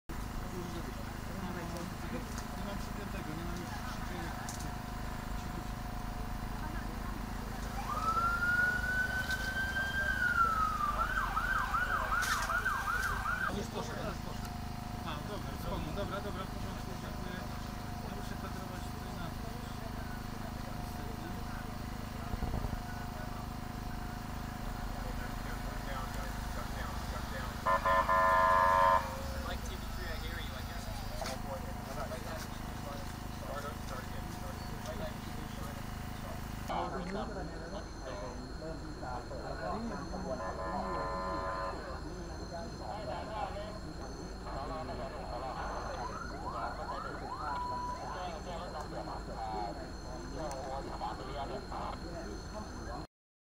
Two Thai ambulances leave the cave rescue site
More videos on Rappler Follow Rappler for the latest news in the Philippines and across the globe. Two ambulances leave the rescue camp area where 4 of the 13 people trapped at the bottom of a cave in Thailand have been rescued.